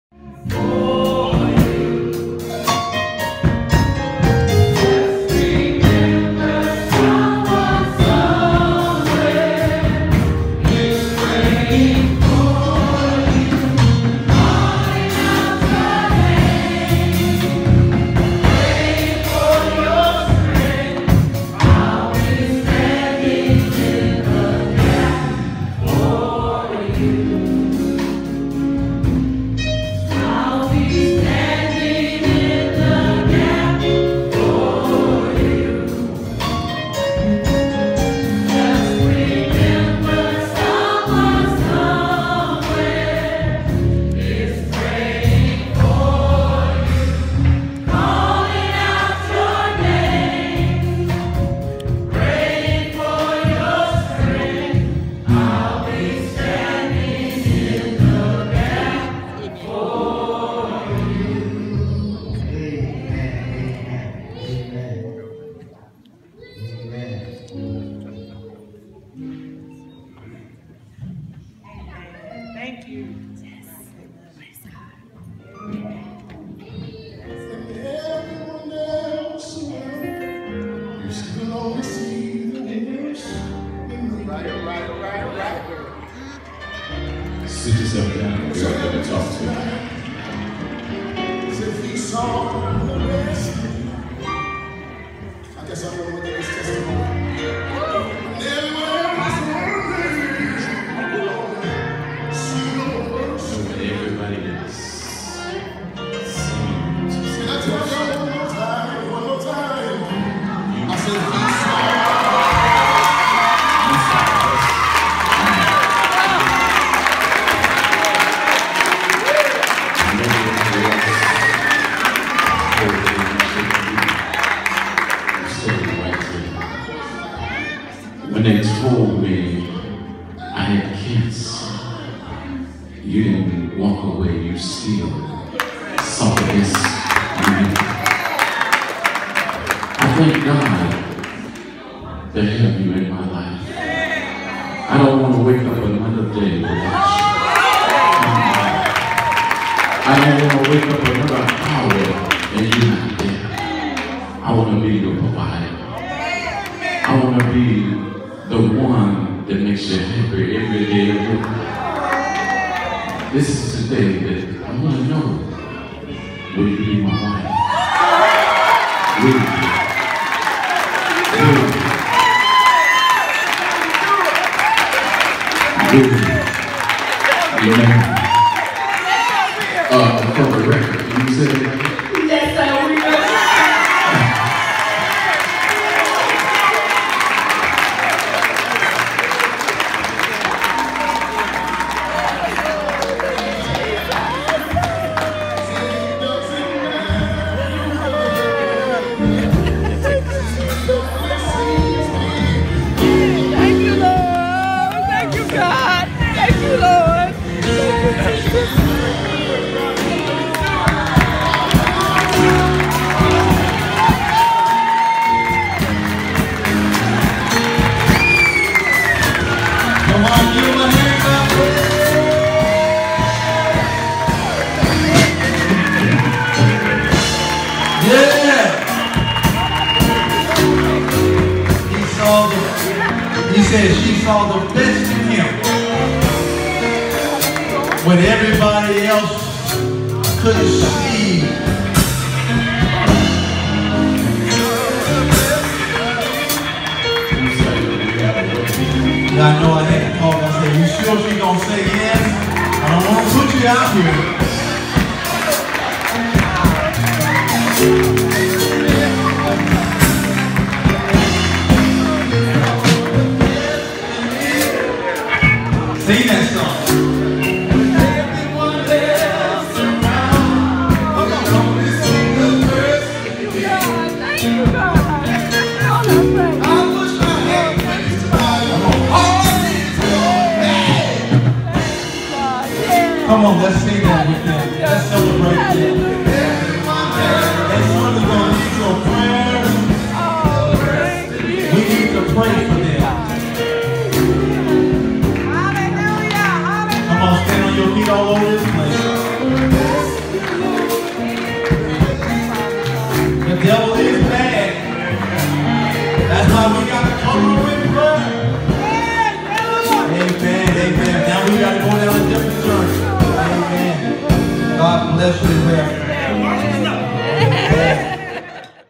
Wedding Proposal @ New Salem M. B. Church
Every real woman would love for her knight and shining armor to come and sweep her off her feet... Well we captured it today! Will you "Marry Me," blessed the entire church! Just watch, Like👍🏽, pray and share it!
It's awesome!
Stand in the Gap for Rev. David Nelson and Vivan Tucker.